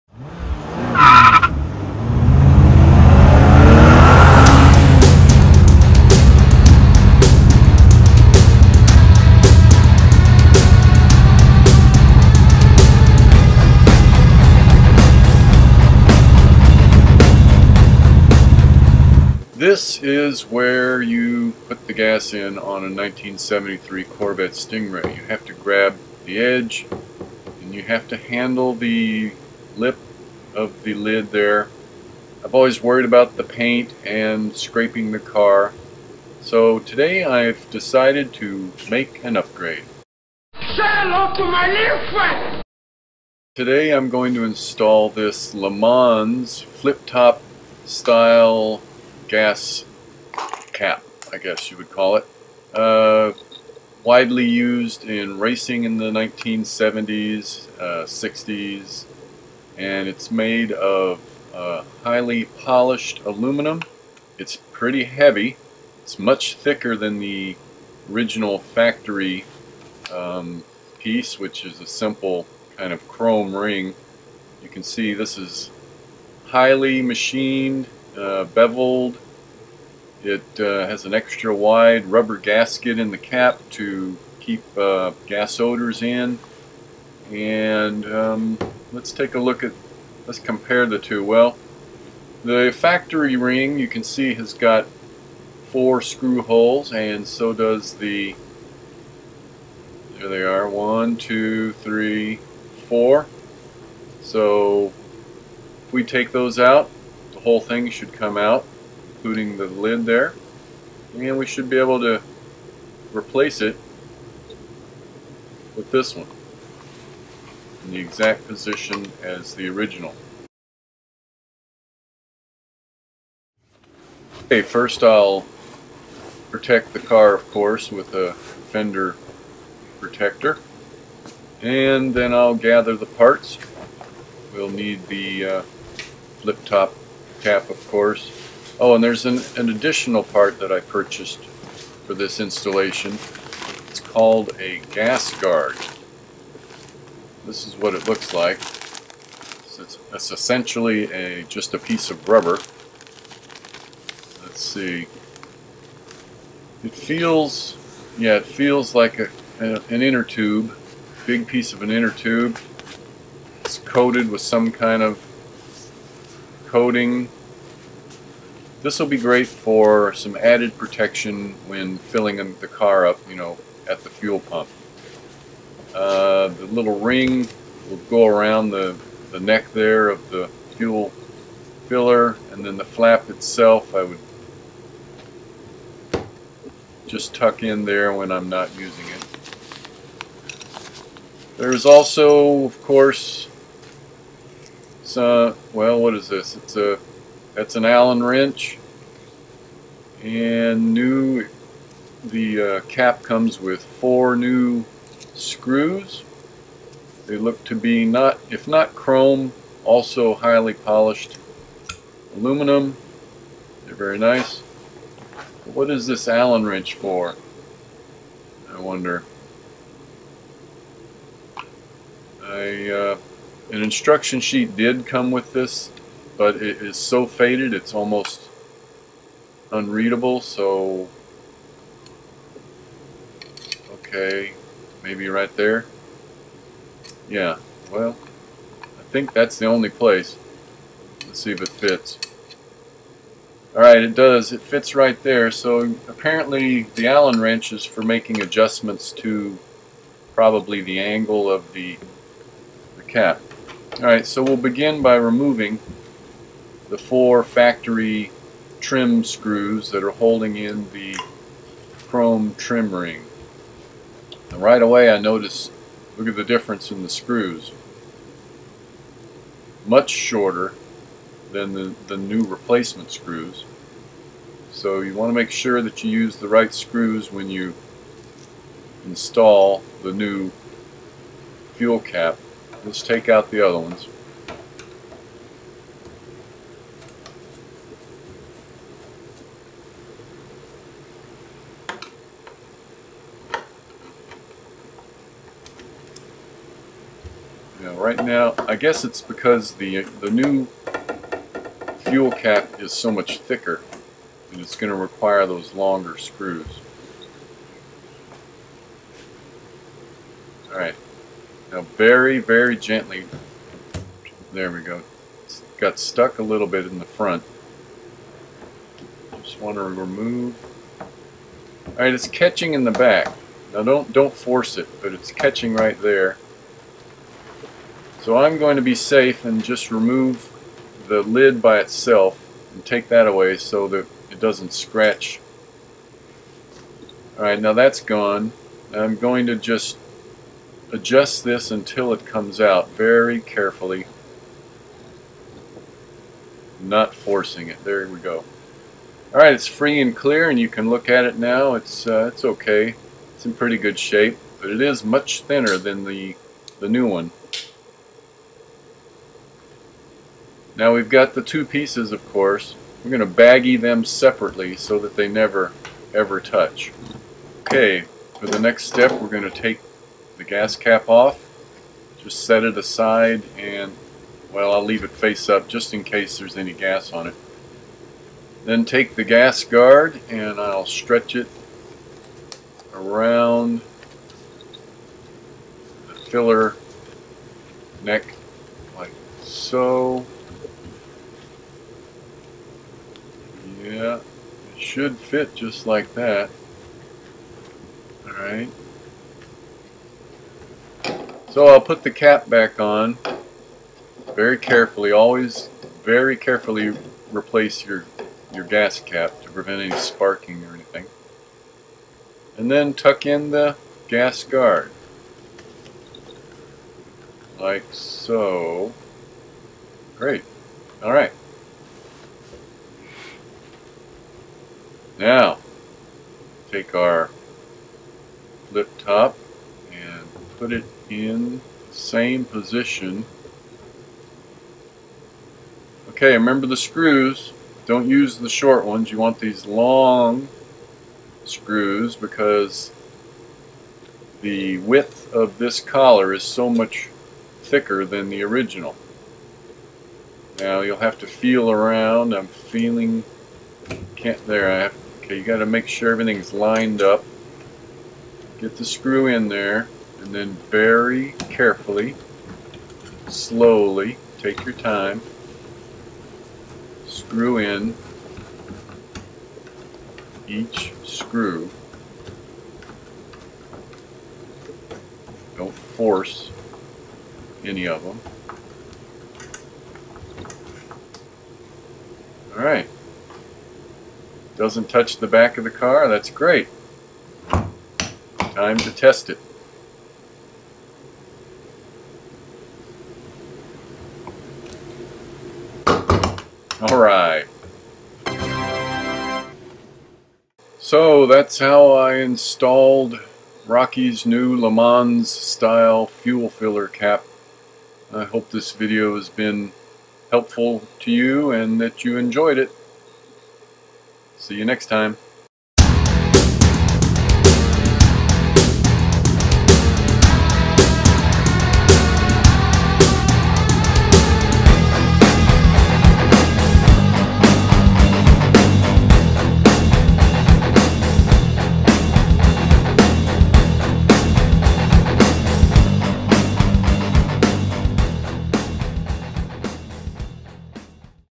My Life With Rokke Ep. 7: "FLIP-TOP FUEL CAP"
How to install a "Le Mans" style flip-top fuel cap on a 1973 Corvette Stingray.